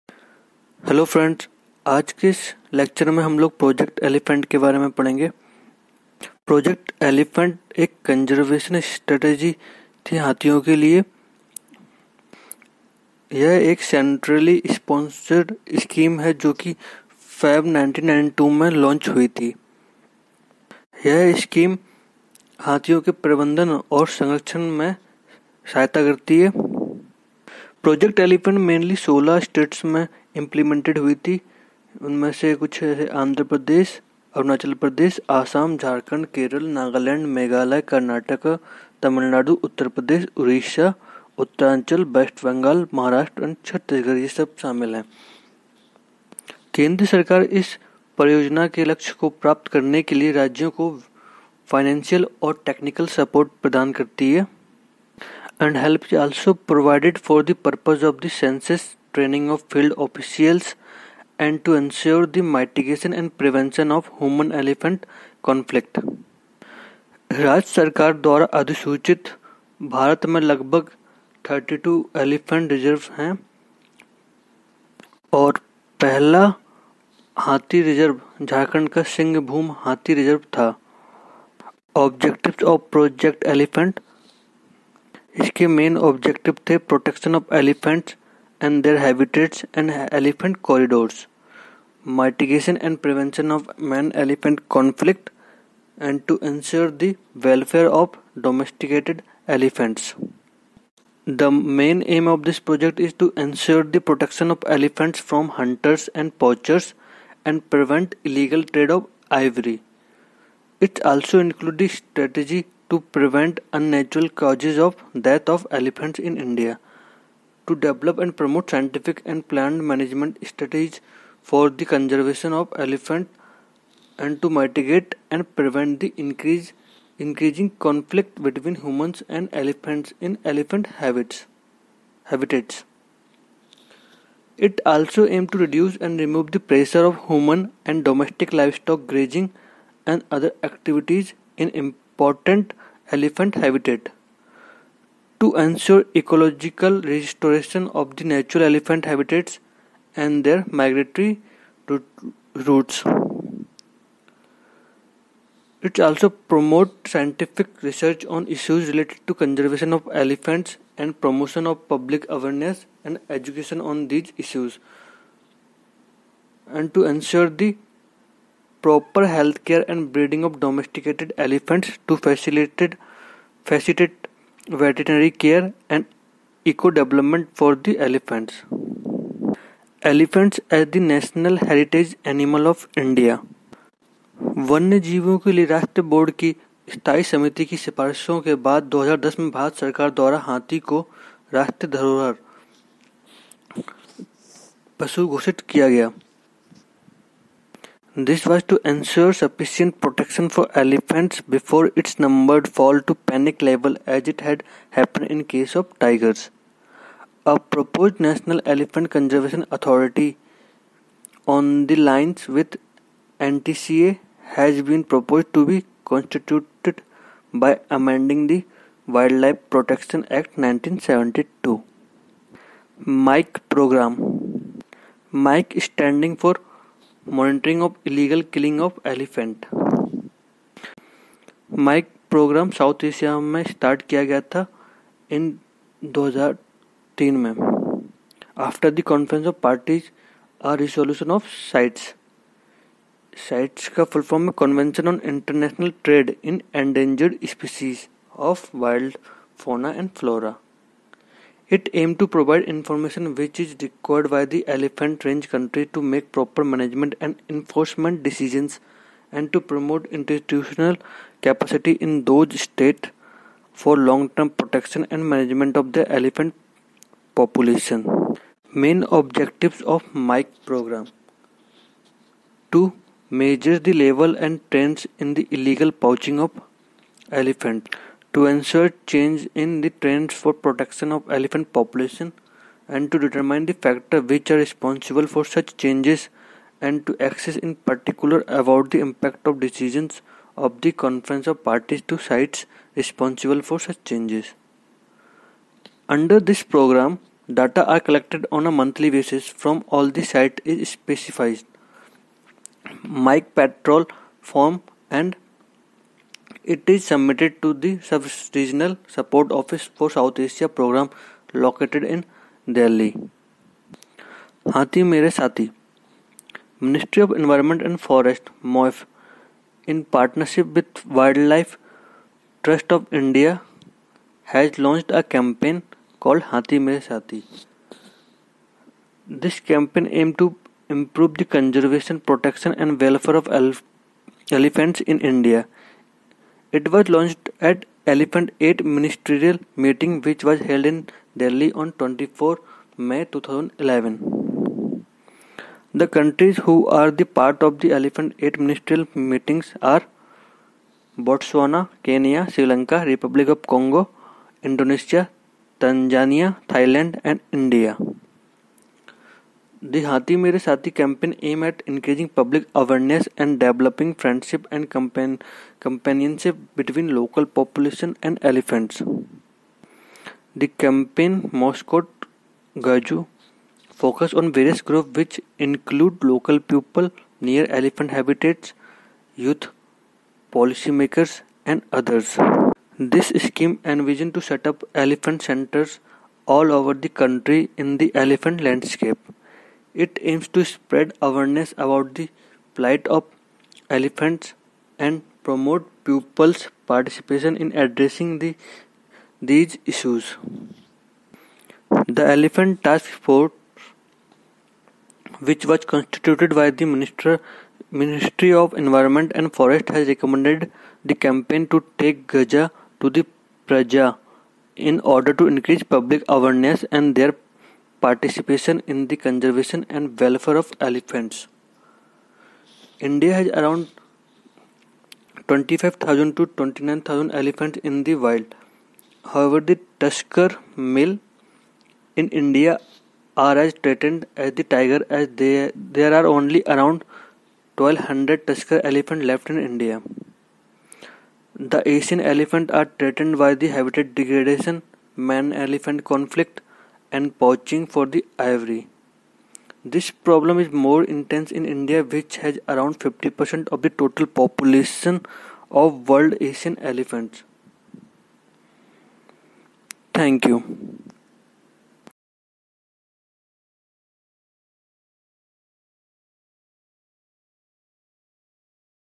Project Elephant | Elephant Reserves | Wildlife Conservation | Conservation efforts in India
this video contains information on Project Elephant and all important conservation measures related to protection and welfare of elephants such as MIKE (Monitoring of Illegal Killing of Elephants) Program), Haathi mere sathi, E-8 ministerial meeting, mascot gaju, etc.

I hope the video will benefit to all the students who are preparing for any kind of competitive examination especially UGC NET, UPSC and State Civil services.